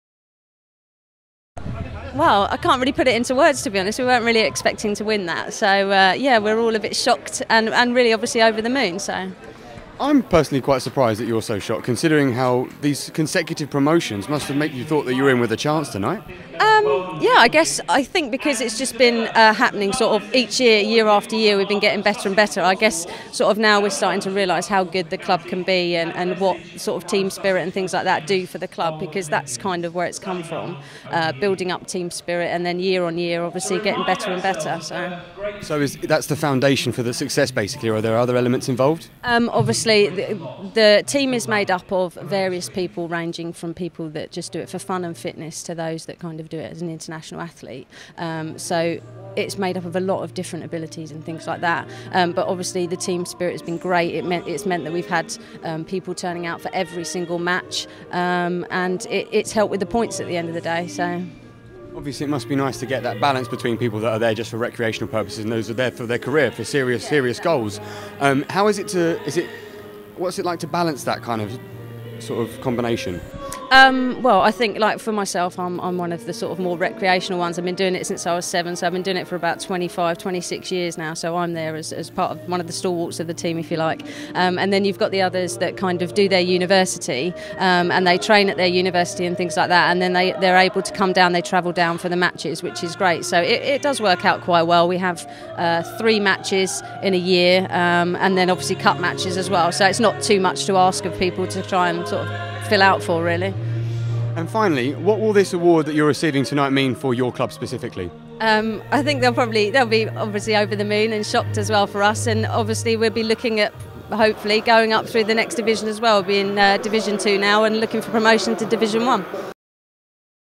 Virgin Holidays Team of the Year - Crawley Athletics Club, Senior Ladies Team
The Crawley Ladies Athletics team have had an amazing track season in 2011. After back to back promotions in 2009 and 2010, for the first time in their history they competed in the UK Women's League in 2011 -- the only ladies club in Sussex to compete at this level.  The ladies are an extremely hardworking bunch, who's team spirit is truly infectious. In July, the team also qualified for the National Super cup final and came out on top in their first year in competition. The ladies have proven they are more than just a successful Sussex team. They are friends and -- perhaps most importantly -- role models to the younger athletes in the club.